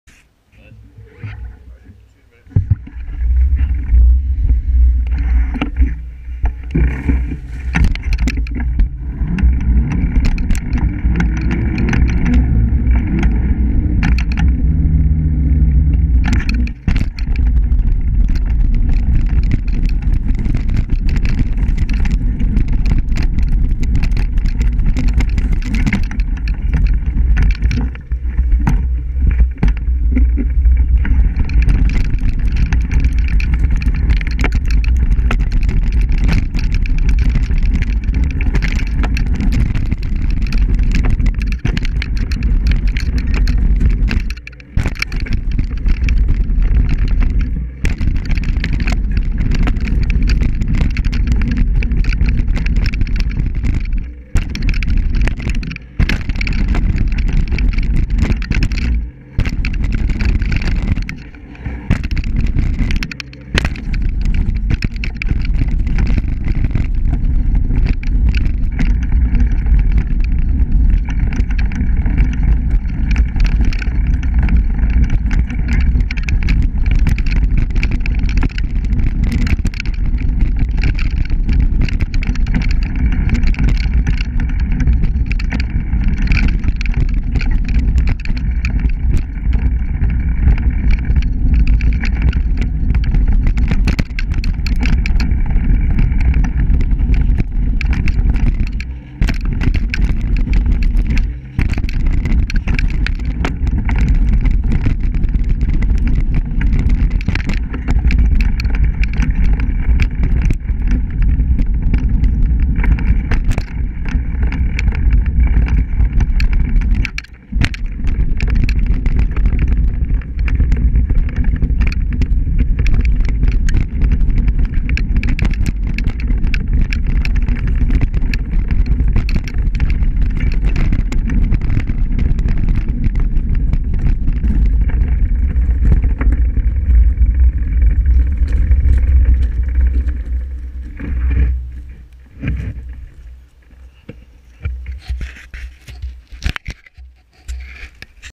Pat hitting the trail
Pat going down a trail by the house